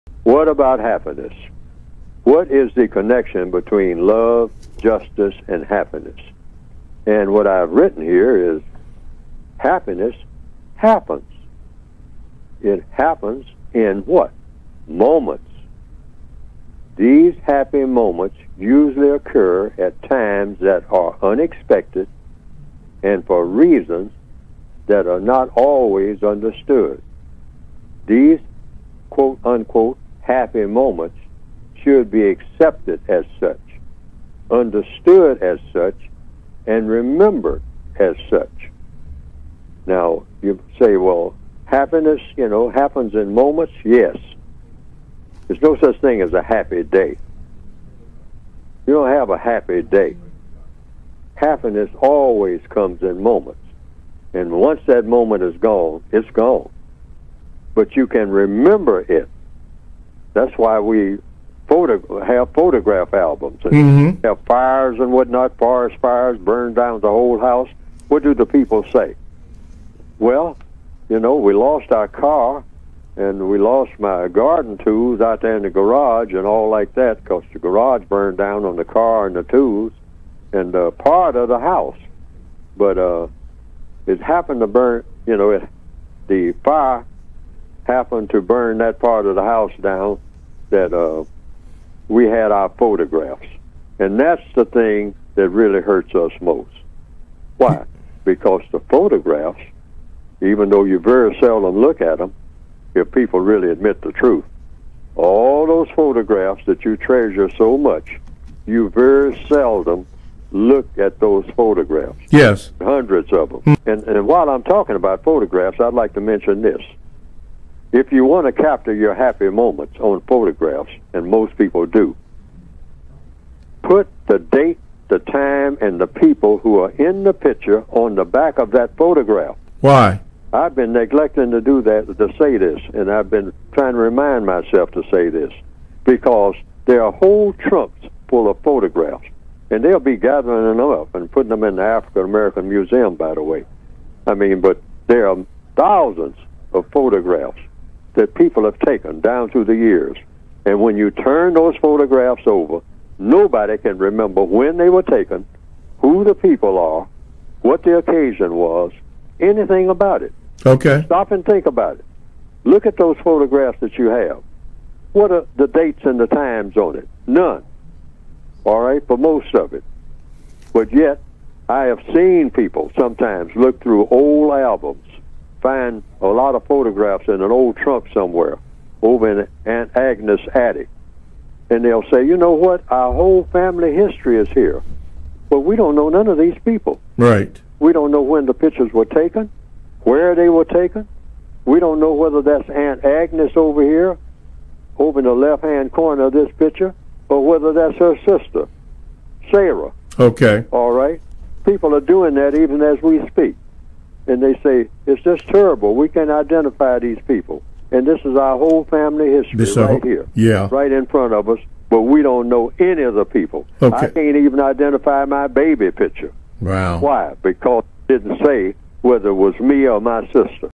Neely Fuller Jr- Happiness And Neglecting To Date/Time Photographs
Neely Fuller Jr explaining how happiness happens in moments and why it is important to date/time photographs.